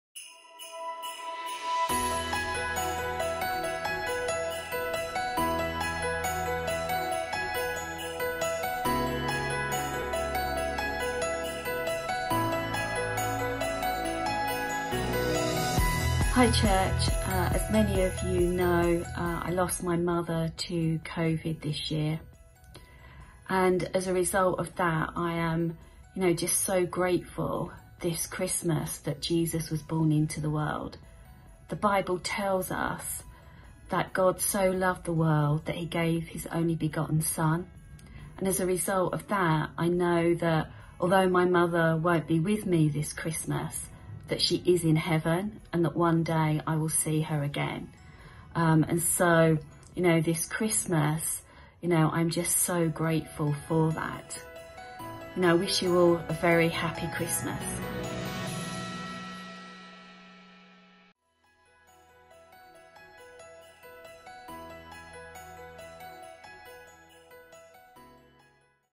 Reason for the Season: A Christmas Thought - Day 7
The festive season is upon us; and as we look back at an extraordinary year, we also look forward to the joy and celebration that comes with Christmas.